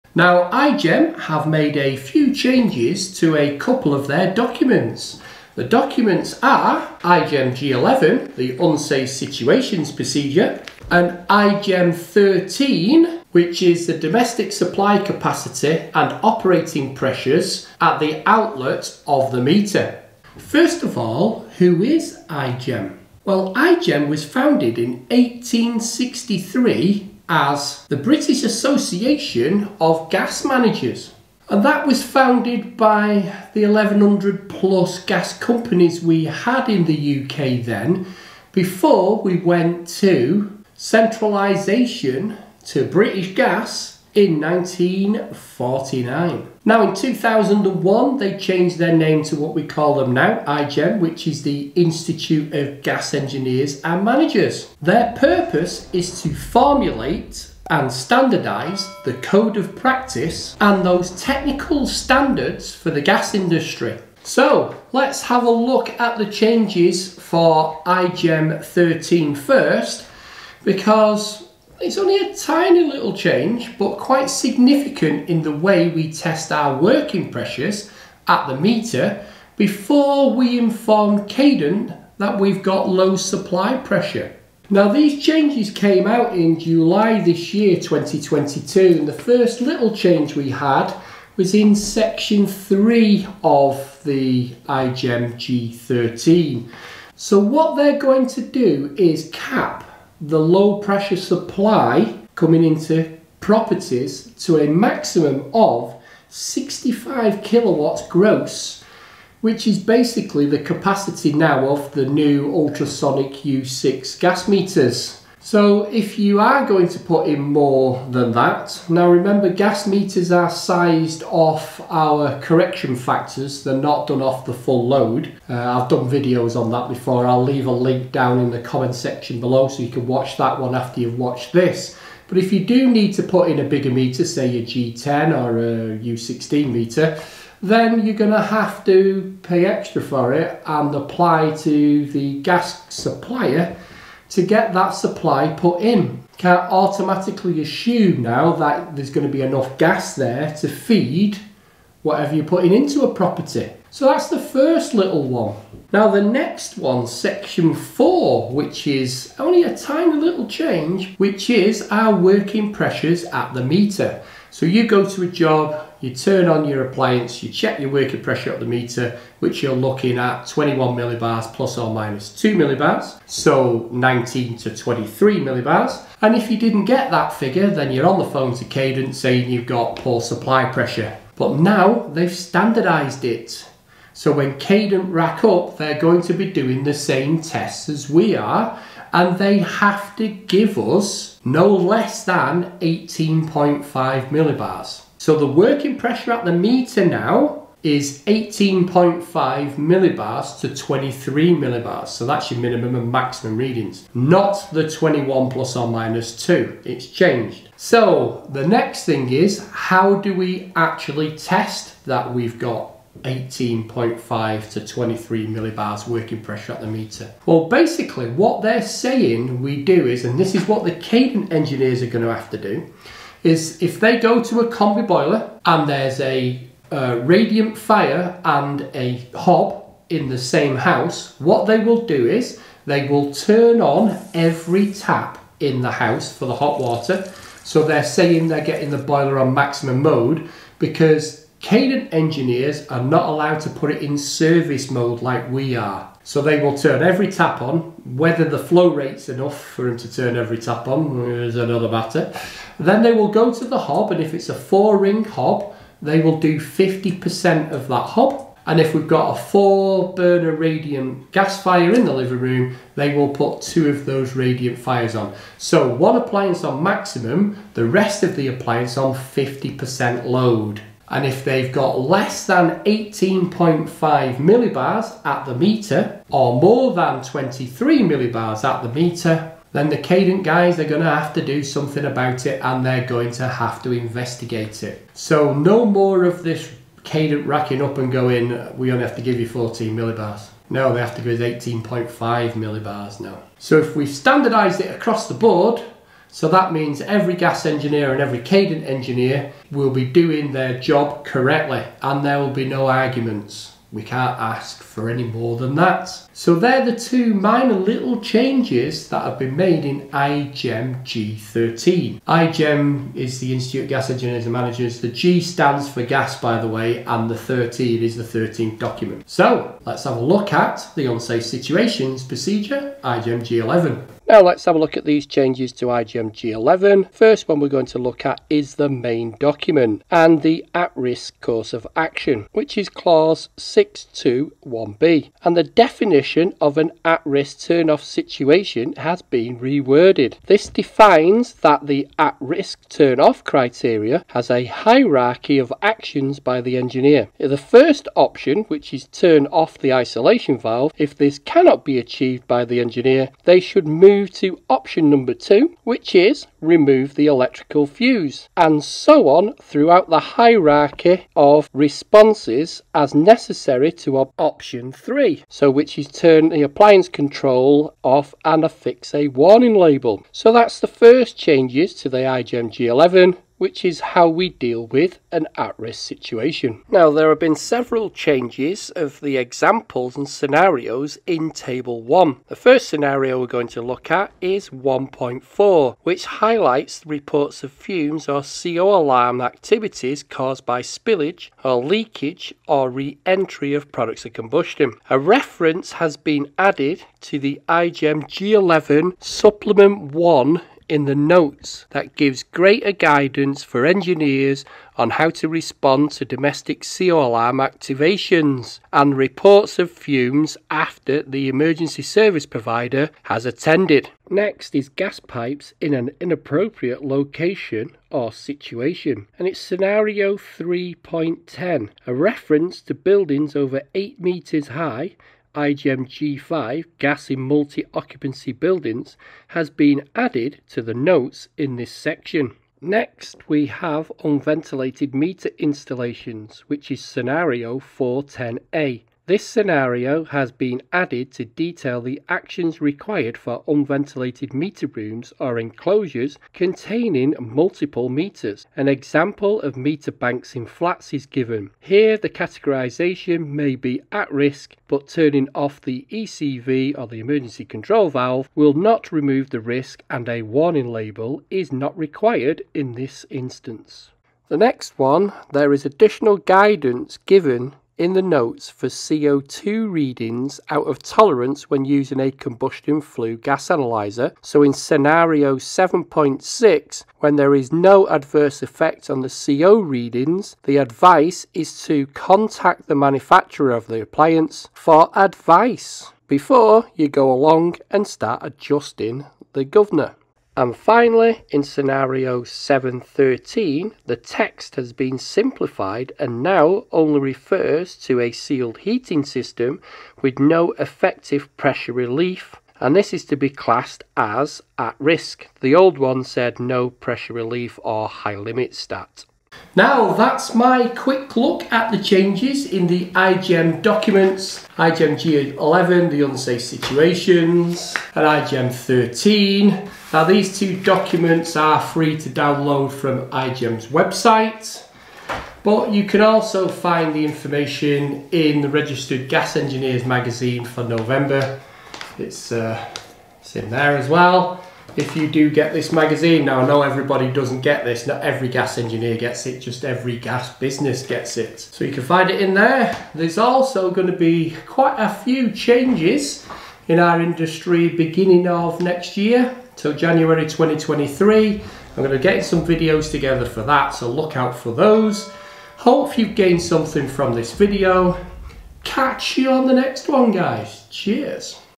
The changes to igem g 11 and 13 that all gas engineers and trainees need to know.
Derek looks at the changes made to igem g 11 and 13. He talks us through the new procedure for working pressure at the gas meter and the changes made to the gas industry unsafe situations procedure.

Link to sizing a gas meter